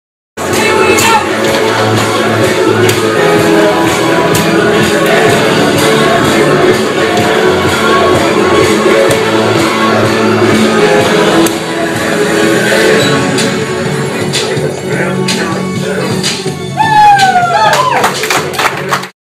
Fred Astaire Dance Studio Twitter flash mob
Fred Astaire Dance Studio San Diego is proud to participate in the Step Up Revolution Twitter flash mob!